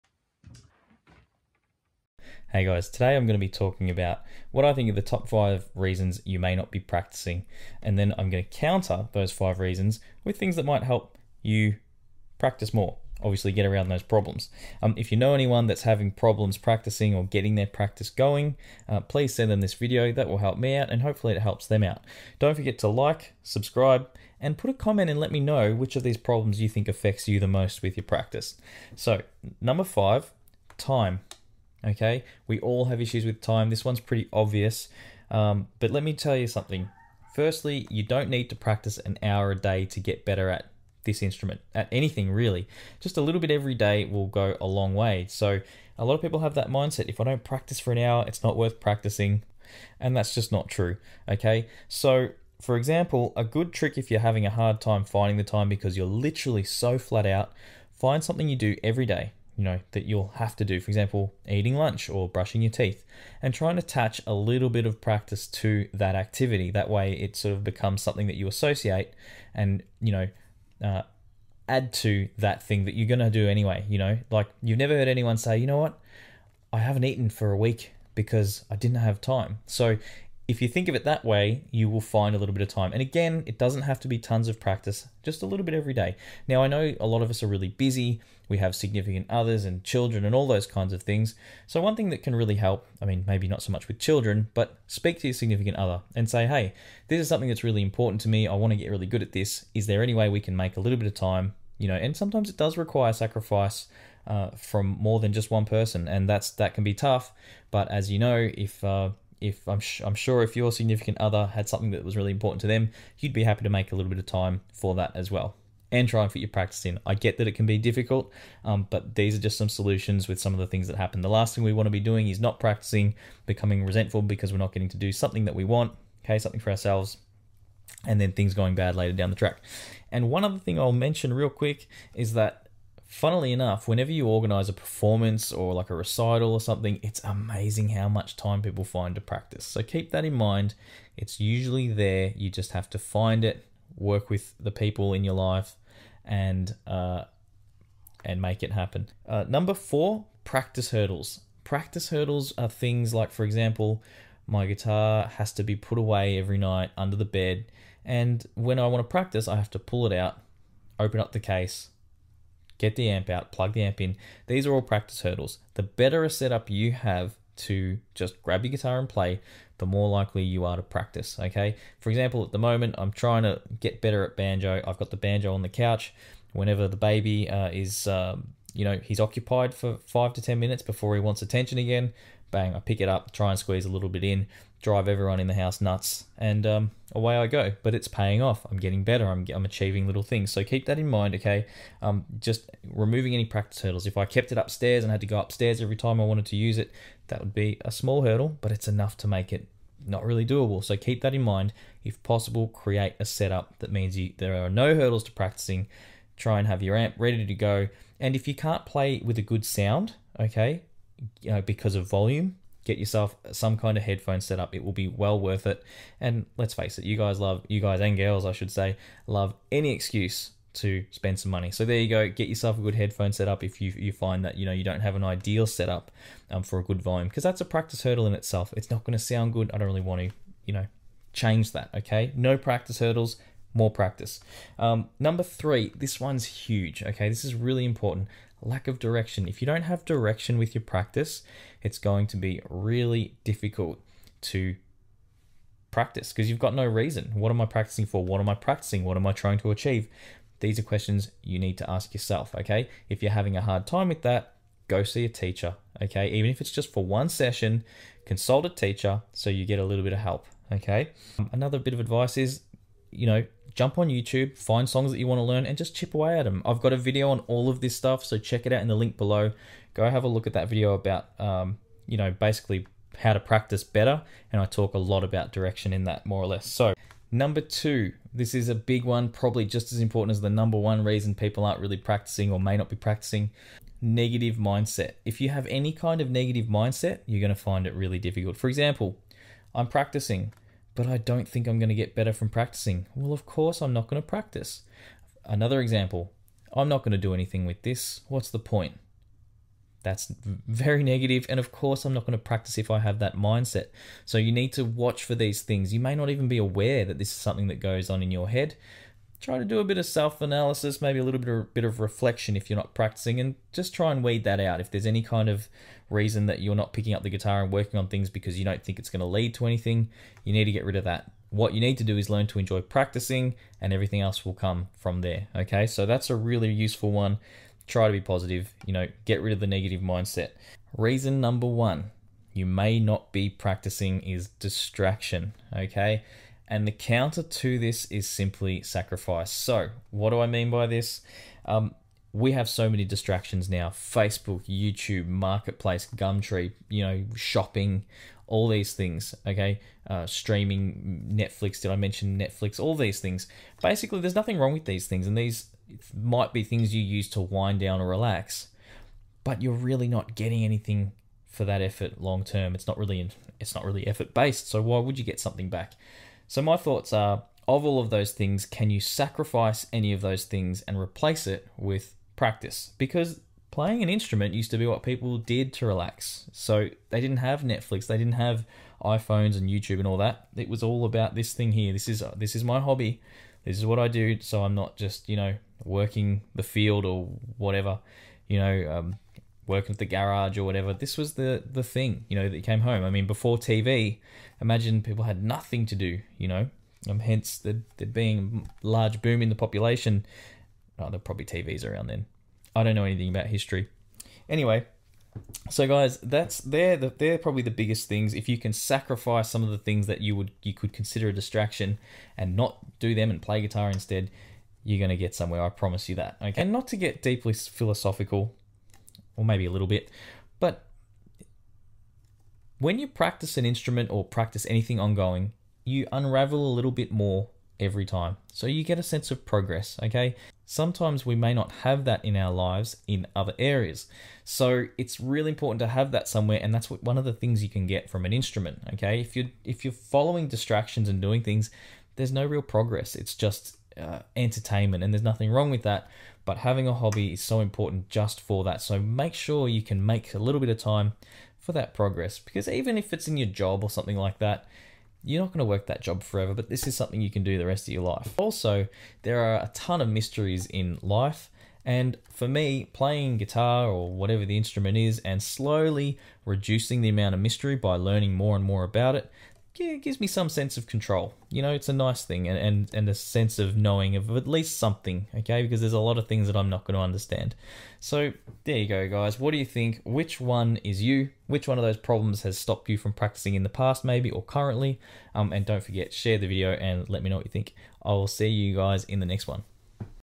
5 Reasons you’re NOT practicing and how to FIX it!
5 reasons you’re not practicing and how to practice more! Which one are you?

Practice Better in 2021

Gretsch 6120 DSW
Mystery Brain Echo
Mooer Radar
Rode NT1
Mbox
Garageband

Checkout my psychobilly band on all streaming services,